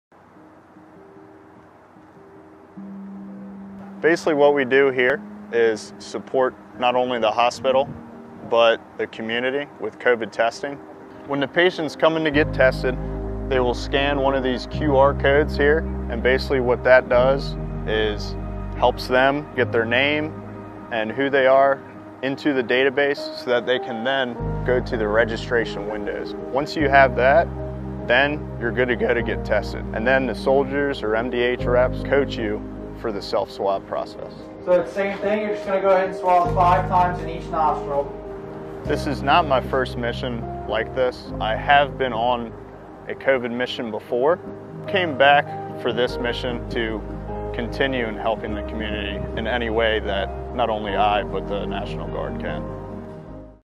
National Guard help at Johns Hopkins COVID site
Members from the Maryland National Guard assisted at a COVID-19 testing site at the Johns Hopkins Bayview Medical Center on Jan. 25, 2022, in Baltimore, Md. Watch this video for details.